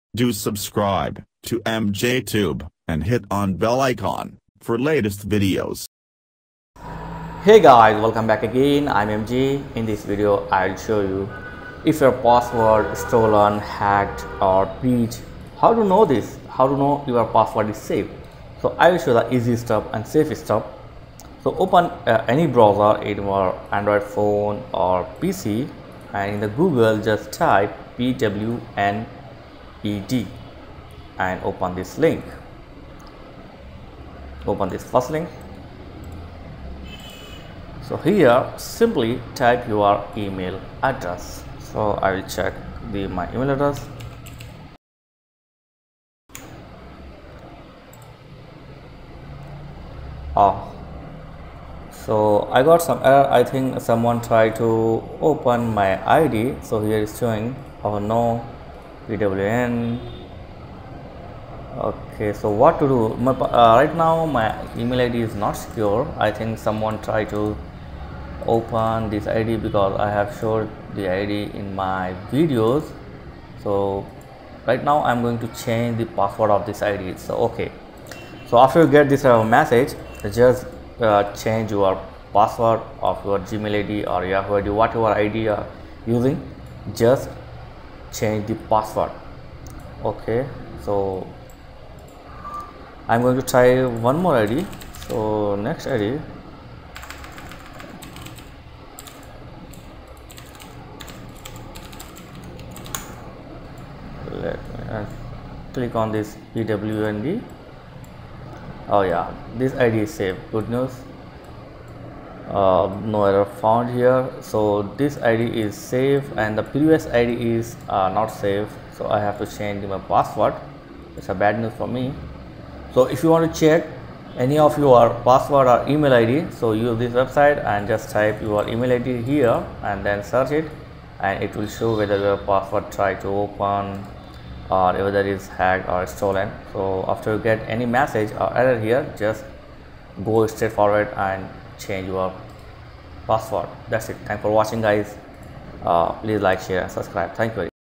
How to Check If your Password Hacked, Stolen & Breach (Easy & Safe)
Know your id and password safe or not